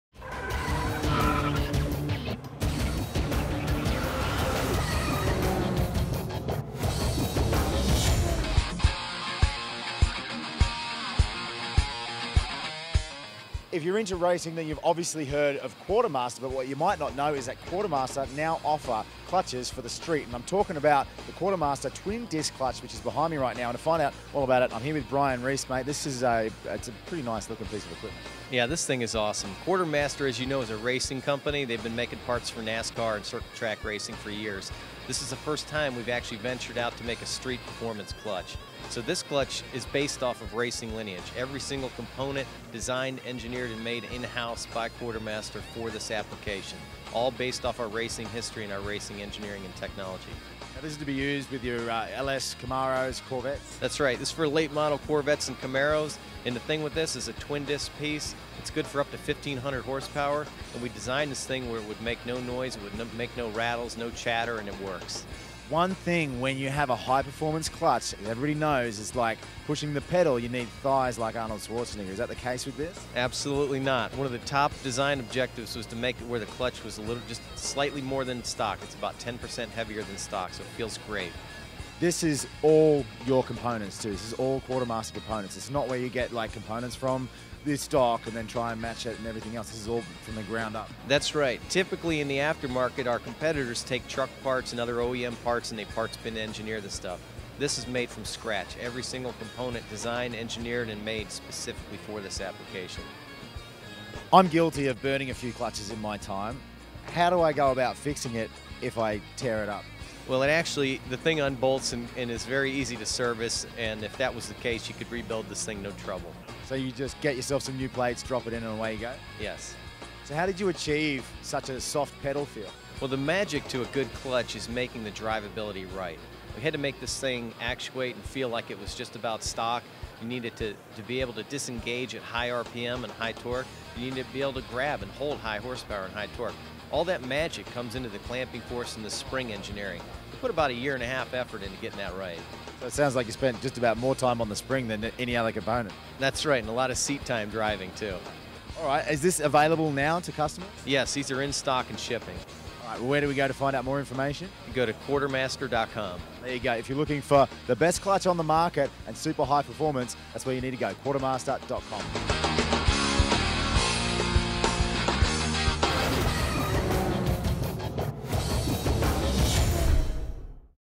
Quartermaster Optimum-SR 10.4" Twin Disc Clutch at SEMA for Camaro, Corvette
Based off of their racing lineage, Quartermaster has launched their street line clutch for the LS platform with this Twin Disc Optimum SR 10.4" clutch, good up to 1,500 horsepower with a soft pedal feel. These are the perfect options for Corvette and Camaro enthusiasts.